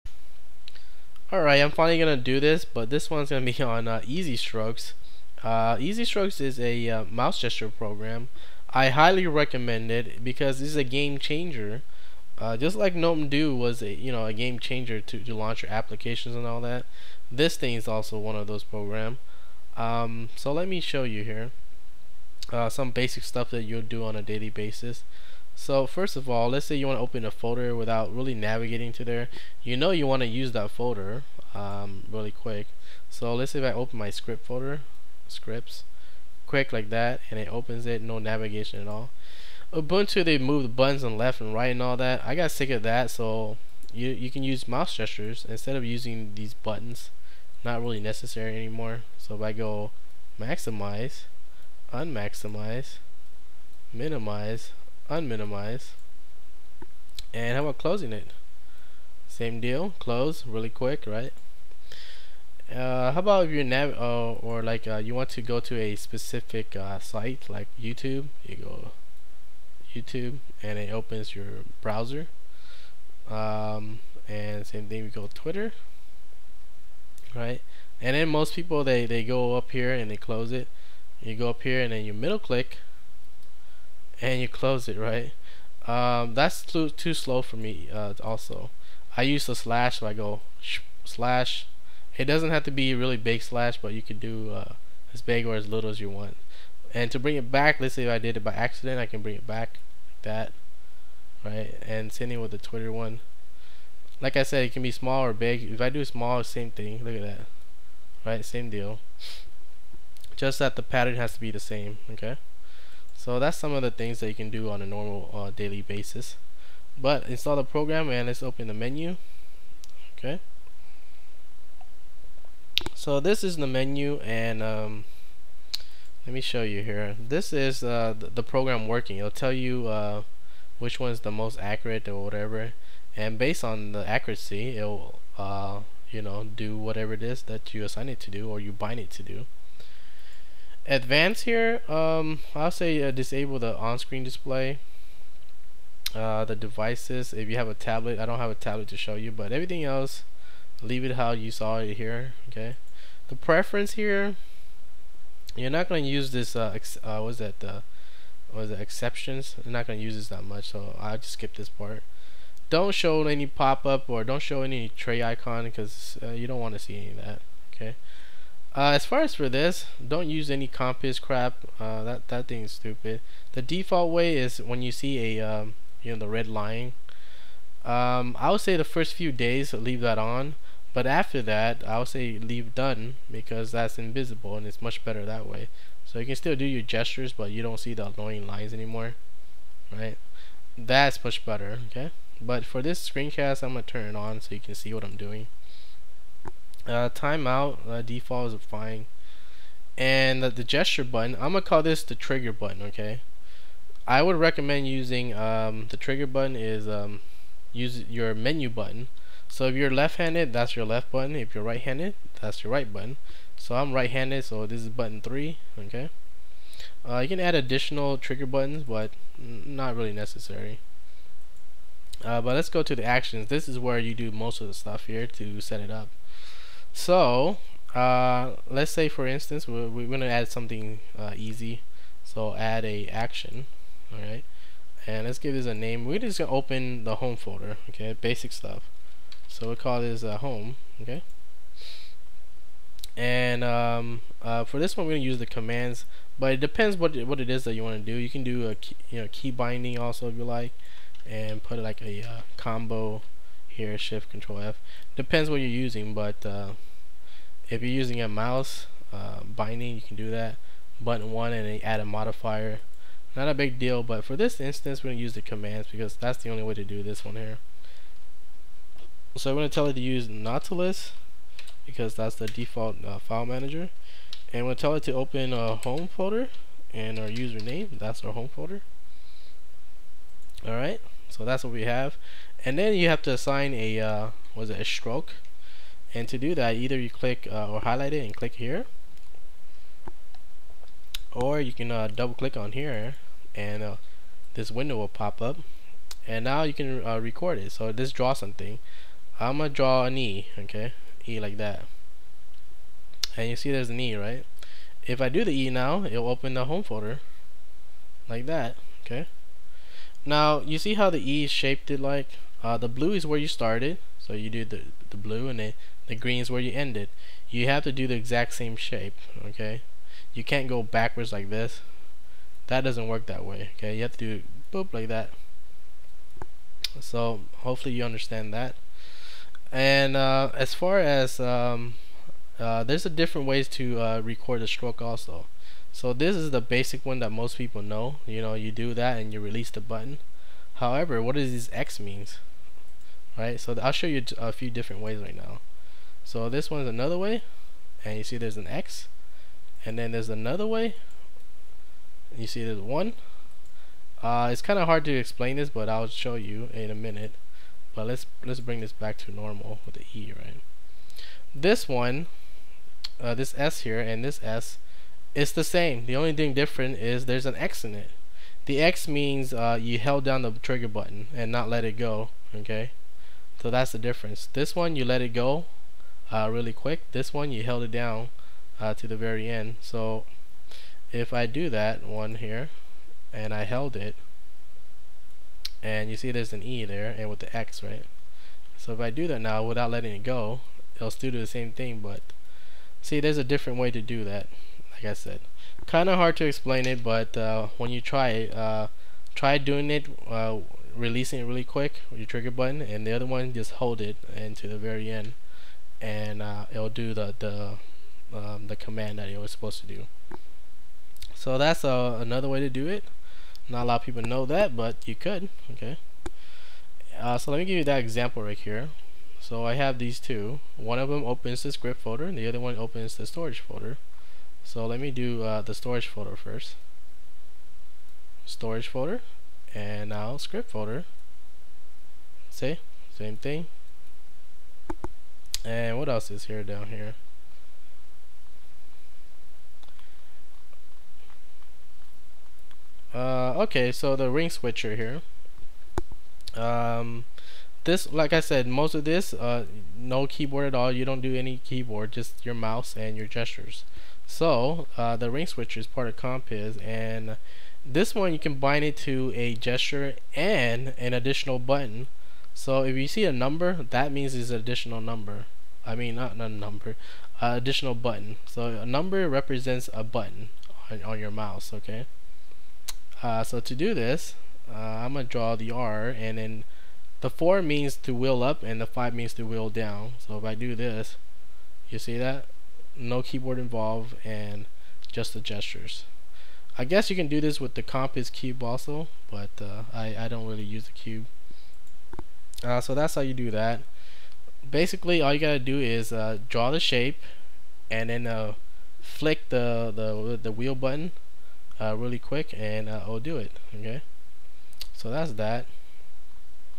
EasyStroke Mouse Gestures Part 1 - Ubuntu 10.04
Easystroke is a gesture-recognition application for X11. It aims to be highly configurable while at the same time providing an intuitive user interface. It was designed with a Tablet PC in mind, but it also works well with a mouse.